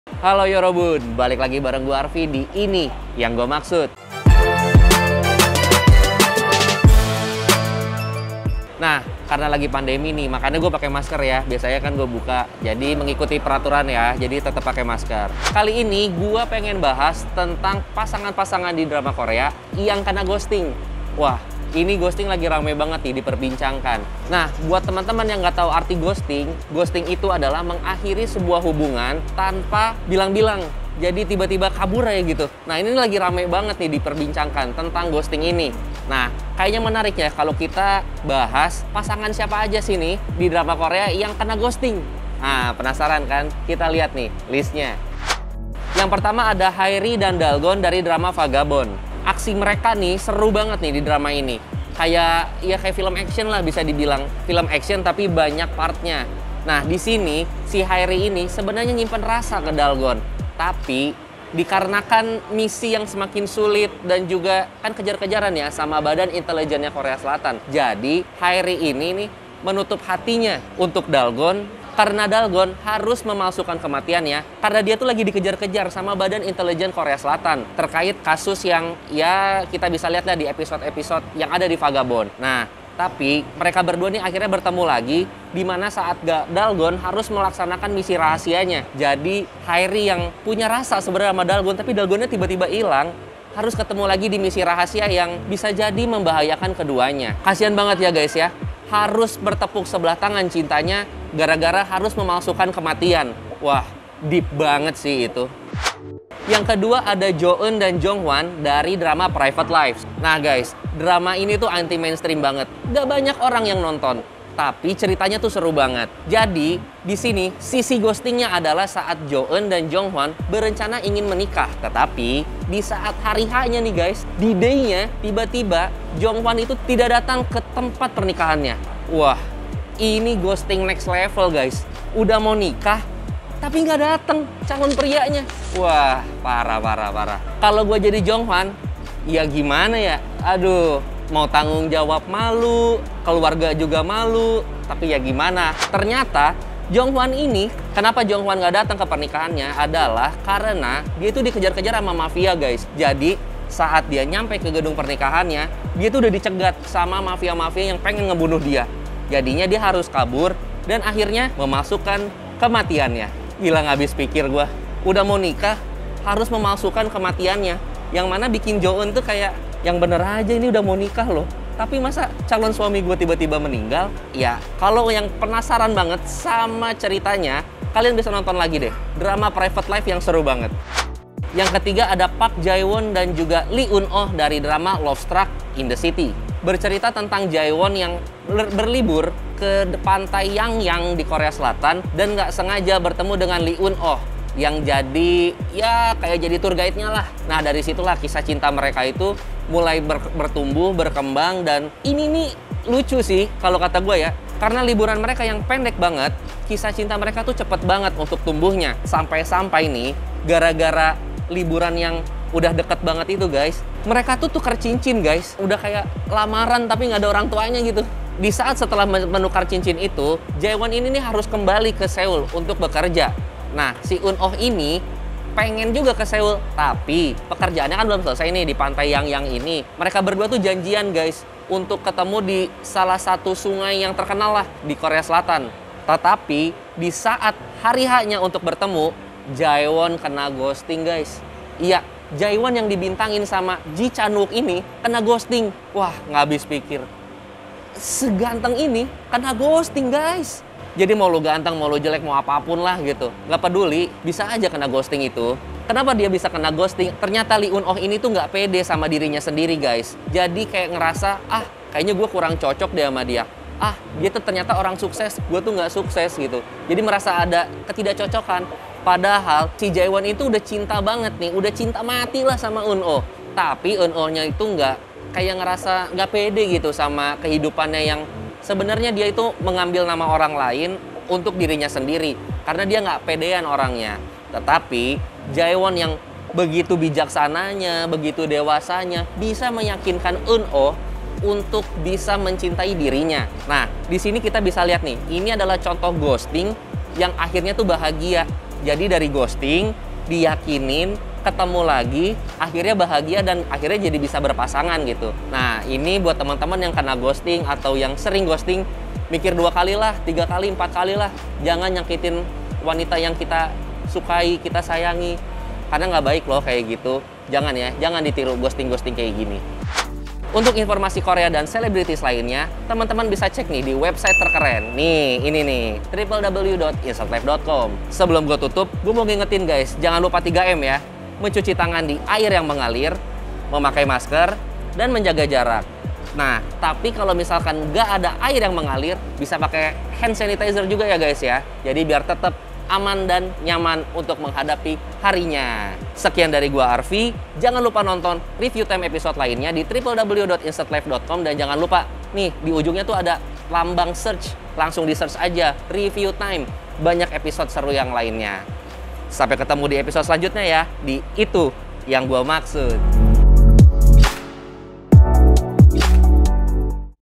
REVIEW TIME | Deretan Couple Drakor Kena Ghosting, Ada Bae Suzy dan Ji Chang Wook
Ghosting, adalah istilah yang sedang gemar digunakan muda-mudi. Memiliki pengertian digantung dalam hubungan, ternyata couple dalam drakor juga banyak yang mengalaminya. Ada yang sedih ada yang bahagia, ada yang marah tetapi ada juga yang senang. Dalam Drakor Vagabond contohnya Bae Suzy harus menahan cintanya terhadap Lee Seung Gi karena ia harus memalsukan kematiannya. Adapun Seohyun dalam drama 'Private Lives' bersama Go Kyung Pyo harus rela pernikahannya tidak jadi karena adanya mafia. Lalu yang terakhir ada pasangan Ji Chang Wook dan Kim Ji Won yang cocok sekali tetapi cinta mereka tidak bisa bersatu. Review Time akan bahas sedetail mungkin dengan pembawaan yang serius tapi santai. Bersama host magang yang takut Covid makanya pake masker, tonton dan saksikan hanya di